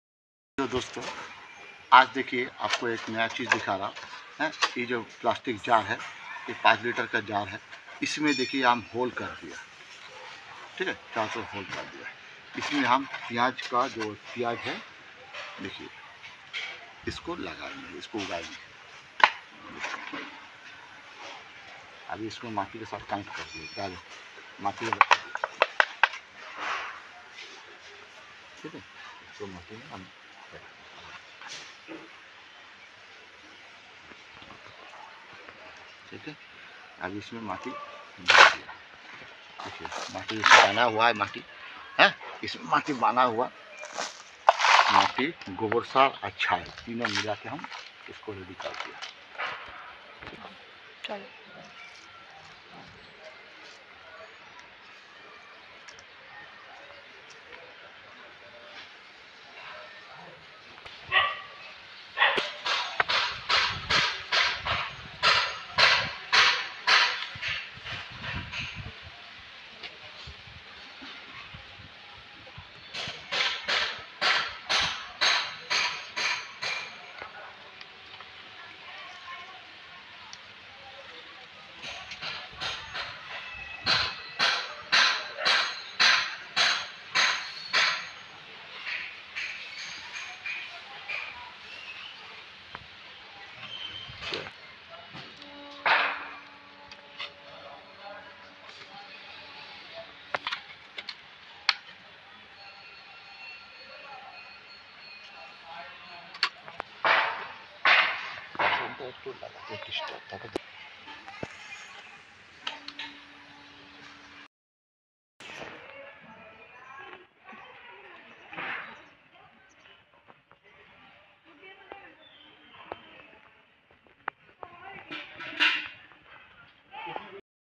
New Style Onion 🧅 Planting// akdom new tarike se piyaj lagana
hello sathio or Dosto ❤️

aj onion lagane ke ak new style dikhaye hai ..